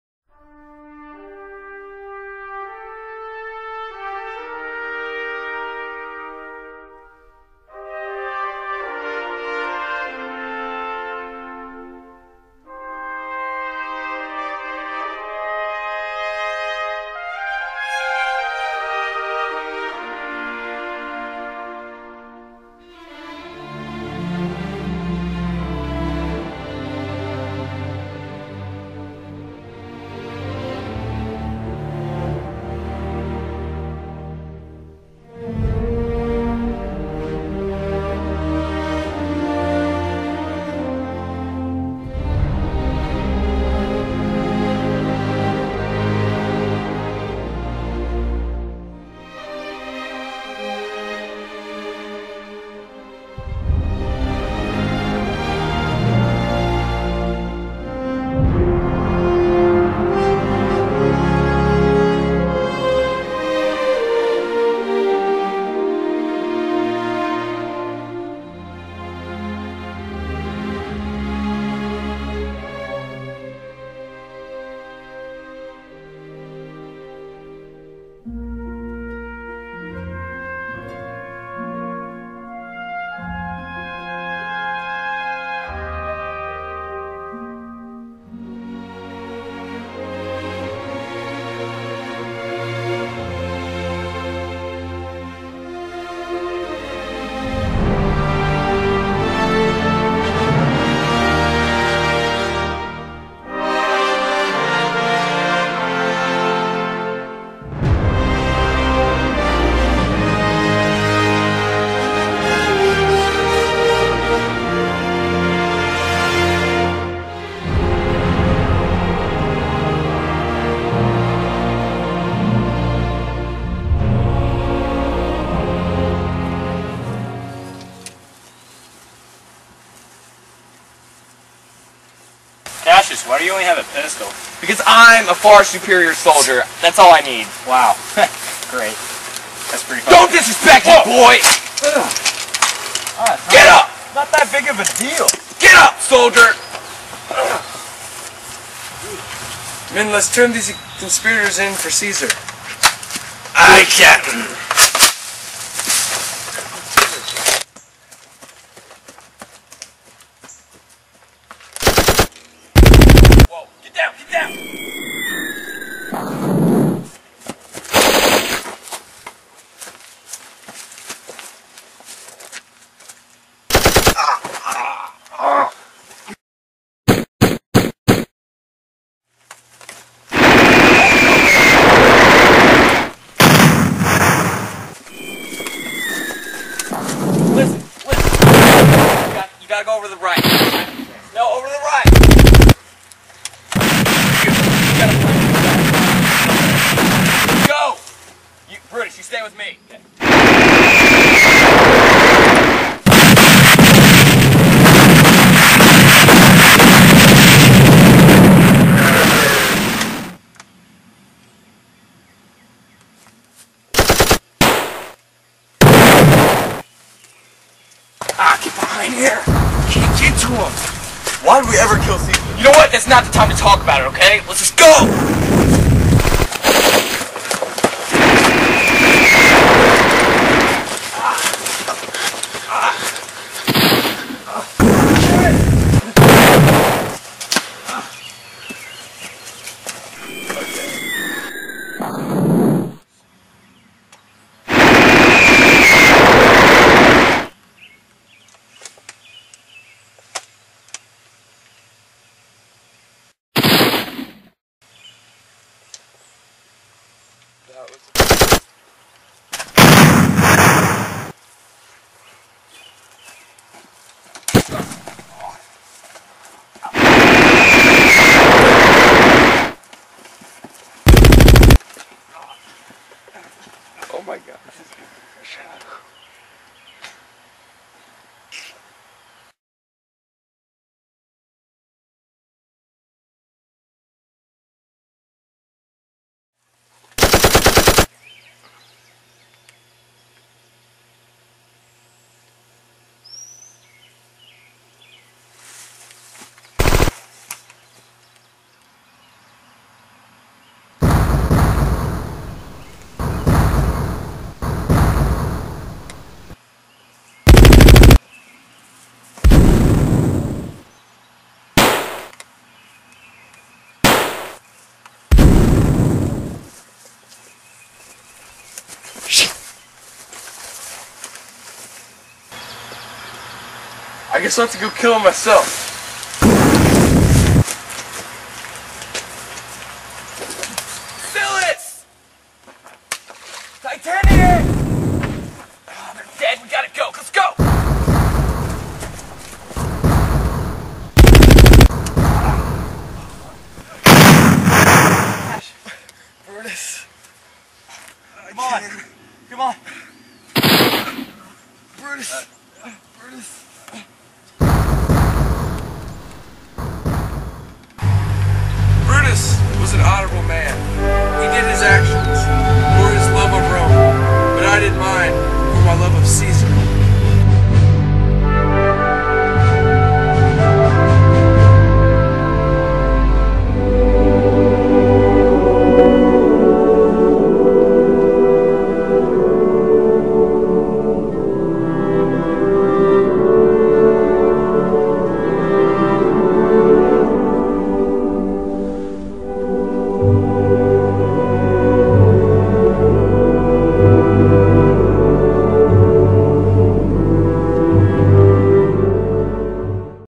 Julius Caesar War Movie
This is based on Act V of the play Julius Caesar.